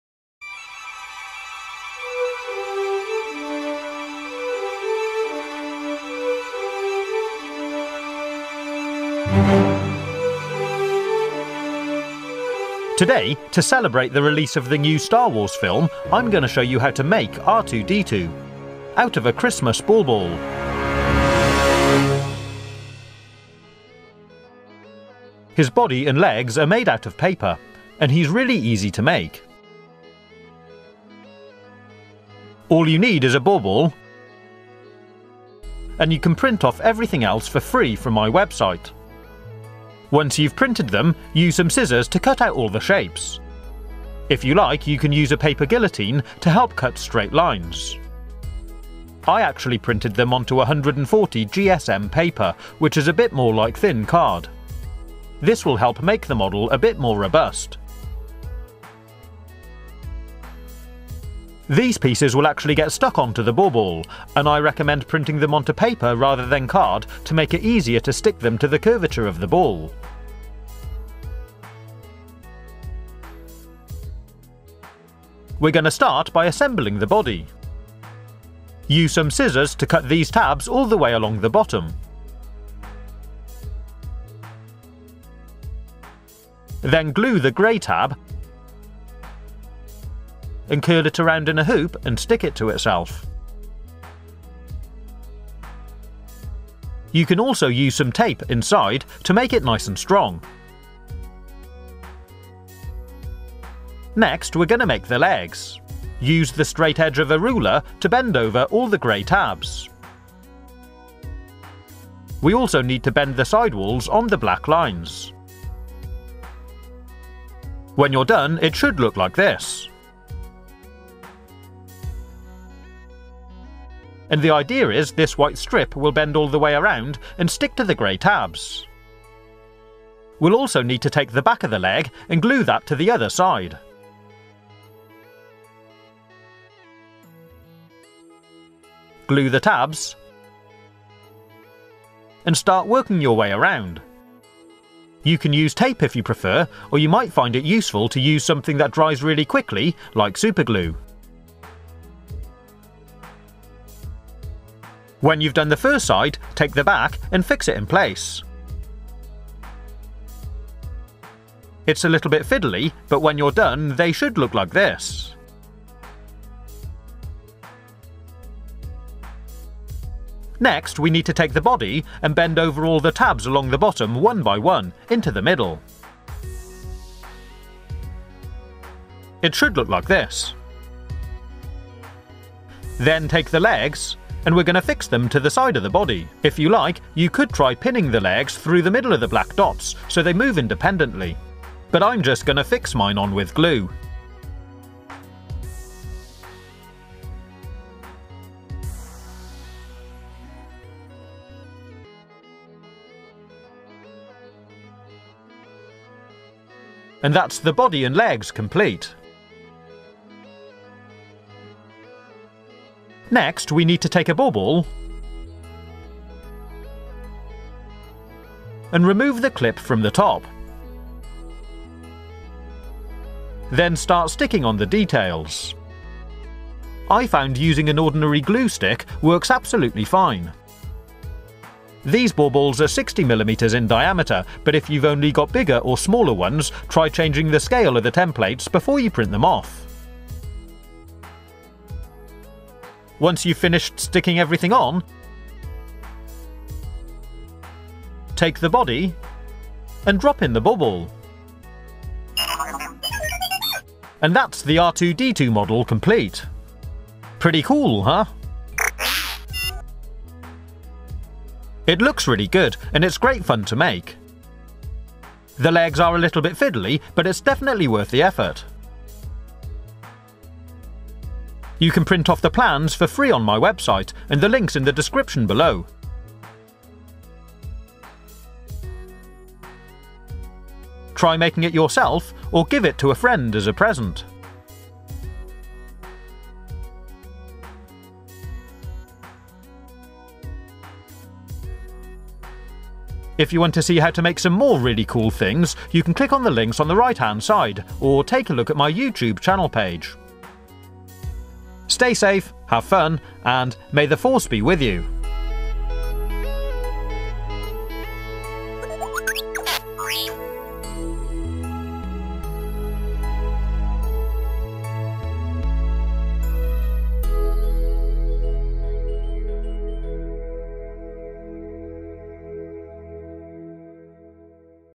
Make R2-D2 Star Wars Model form a Christmas Ornaments
How to make R2-D2 droid from Star Wars. Use paper templates and a Christmas Bauble to make a model of R2-D2. Fun paper craft, just print off the templates. These templates are designed for a 60mm bauble, if you only have larger or smaller baubles try re-scaling the templates before you print them.